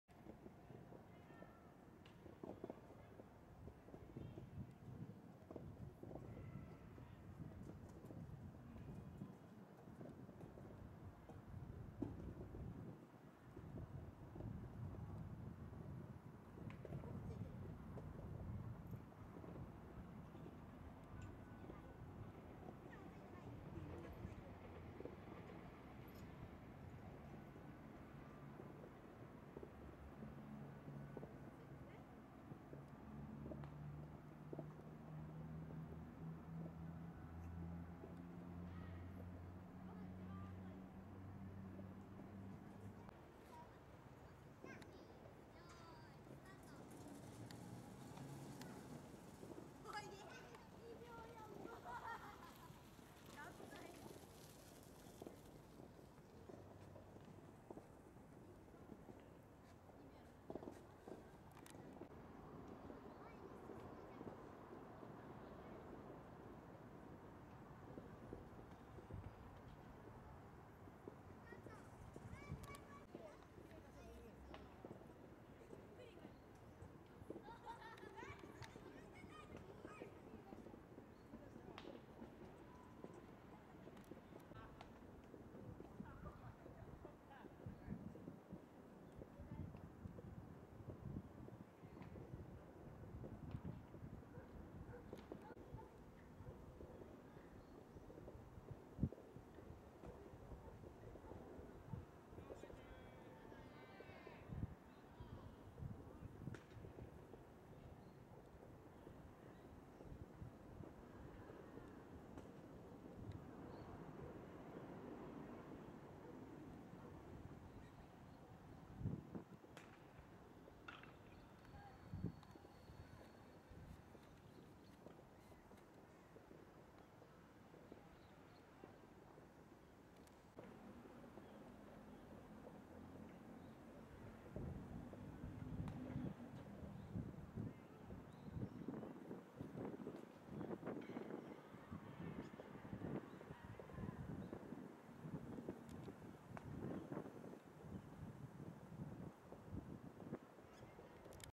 広島西部埋立第二公園【謎の池】
この公園は、昔は海だった所を埋め立てて、出来た公園です。この公園にある池はその海の名残を残していて、池の中央にある小己斐島がそのもの！Googleマップで見ると一目瞭然。おもしろい事に、この池は海水で海と繋がっているので干満に合わせて池の水面が上下します（笑）